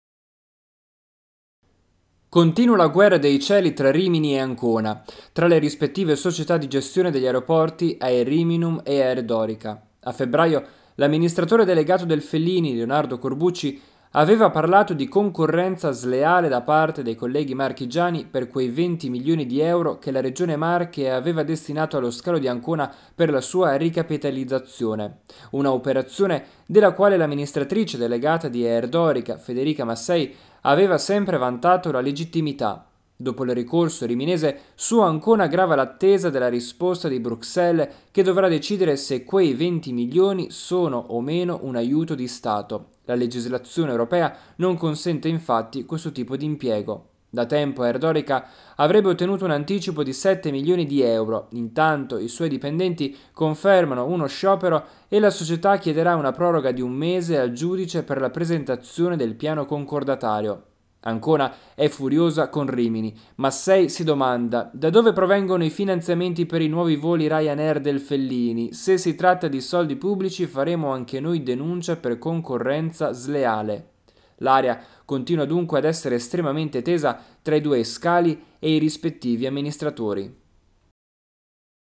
RIMINI: Ancona pronta a denunciare se nuovi voli sono finanziati dal pubblico | VIDEO
Continua la guerra dei cieli tra Rimini e Ancona. Tra le rispettive società di gestione degli aeroporti Airiminum e Aerdorica. A febbraio l’amministratore delegato del Fellini, Leonardo Corbucci aveva parlato di “concorrenza sleale” da parte dei colleghi marchigiani per quei 20 milioni di euro che la Regione Marche aveva destinato allo scalo di Ancona per la sua ricapitalizzazione. Una operazione della quale l’amministratrice delegata di Aerdorica Federica Massei aveva vantato la legittimità. Dopo il ricorso riminese, su Ancona grava l’attesa della risposta di Bruxelles che dovrà decidere se quei 20 milioni sono o meno un aiuto di Stato. La legislazione europea non consente infatti questo tipo di impiego. Da tempo Aerdorica avrebbe ottenuto un anticipo di 7 milioni di euro. Intanto i suoi dipendenti confermano uno sciopero e la società chiederà una proroga di un mese al giudice per la presentazione del piano concordatario.Ancona è furiosa con Rimini. Massei si domanda: “Da dove provengono i finanziamenti per i nuovi voli Rayanair del Fellini? Se si tratta di soldi pubblici faremo anche noi denuncia per concorrenza sleale”. L’aria continua dunque ad essere estremamente tesa tra i due scali e i rispettivi amministratori.